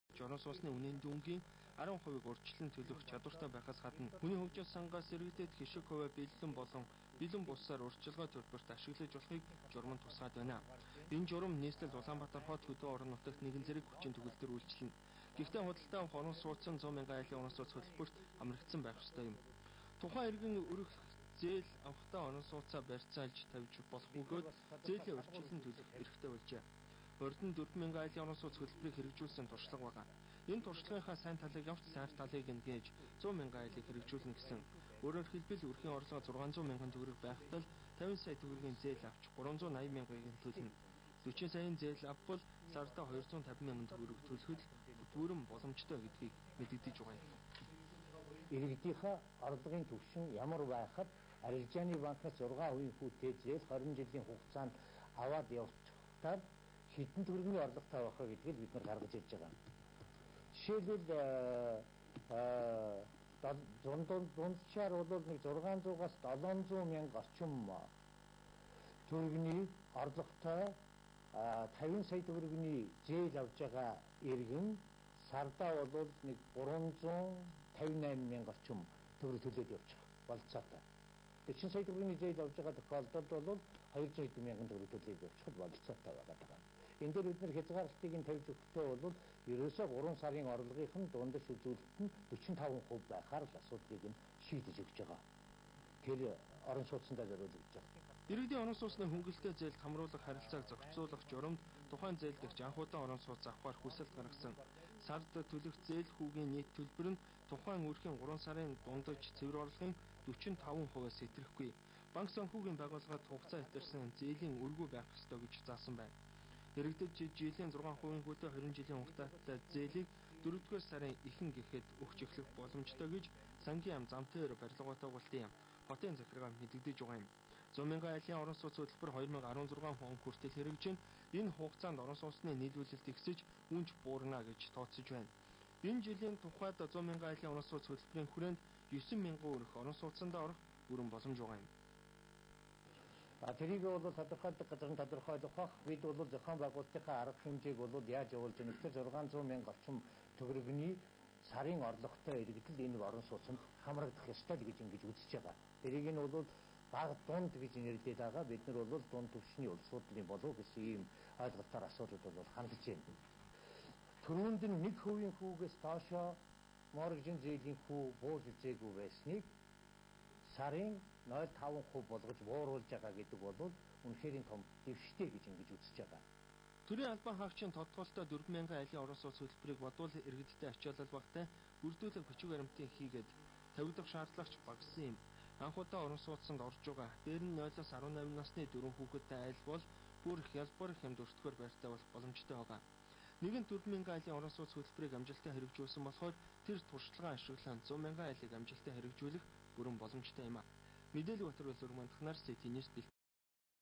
Vip76.mn -City news -Dashdorj Ts -2012-03-01
Зургаан хувийн хүүтэй орон сууцны зээлийн талаар мэдээлэл хийлээ.